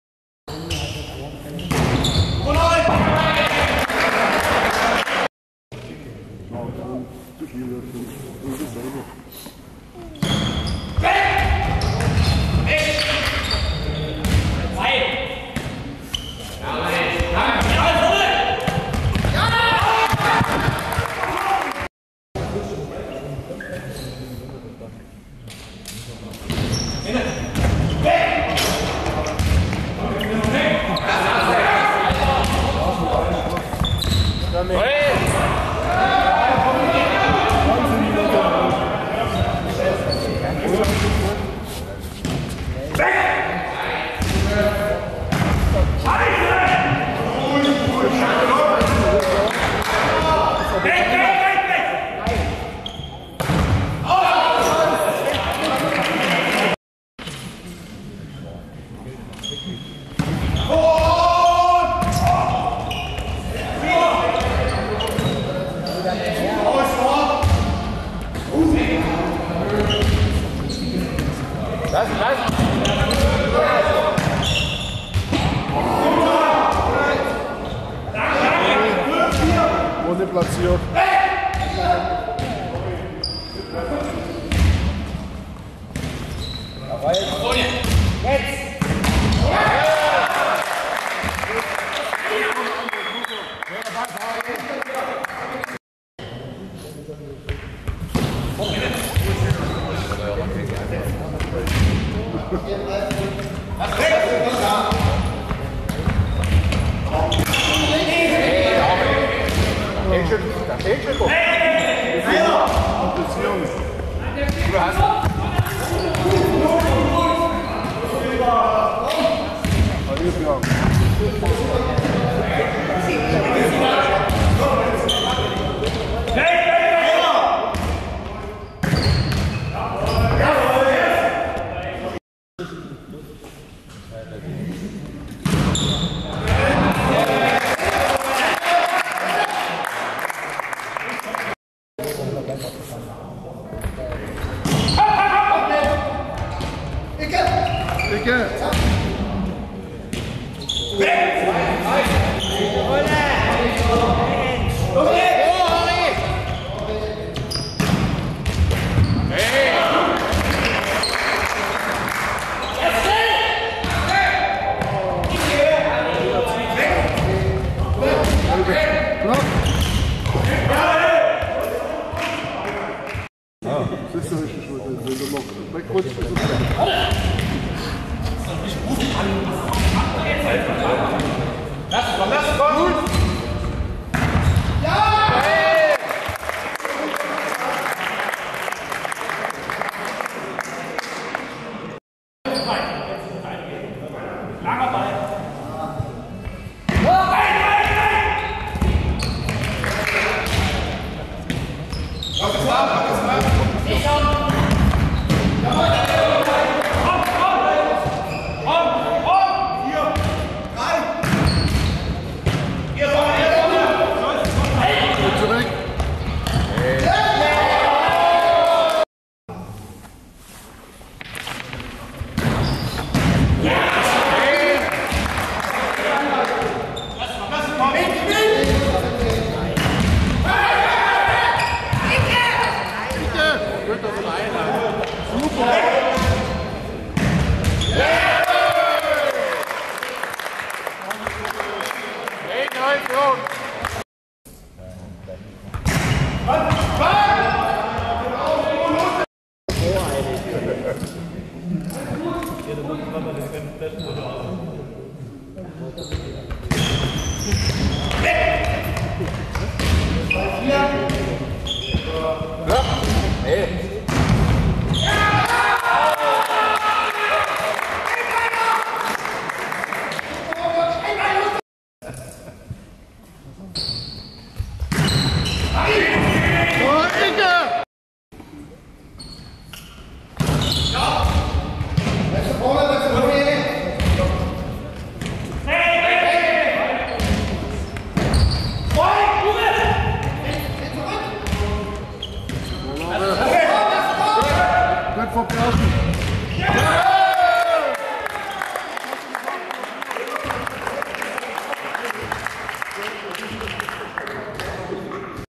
Faustball – Oberliga Sachsen: Heidenau 2. – Kubschütz am 15.11.2015
Im Video seht Ihr Szenen vom 1. Spieltag der Oberliga Sachsen in der Sporthalle an der Grundschule „Bruno Gleißberg“ in Heidenau. Nachfolgend die Ergebnisse von unseren beiden Spielen gegen den SV Kubschütz im Überblick.

SSV Heidenau 2. – SV Kubschütz
1 : 2 (4:11, 12:10, 11:13)
2 : 0 (14:12, 11:8)